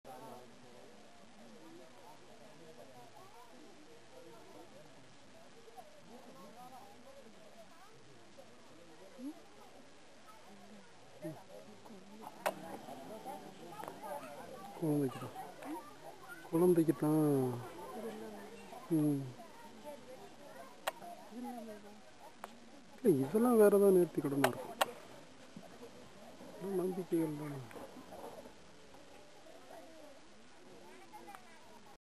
Kattalai mariyamman temple kodanthur cinnar
this temple is inside the forest 3kms from the cinnar tamil nadu  kerala checkpost in udumalai munnar road.people are allowed only on tuesday,thursday.sunday and fullmoondays till 2 pm.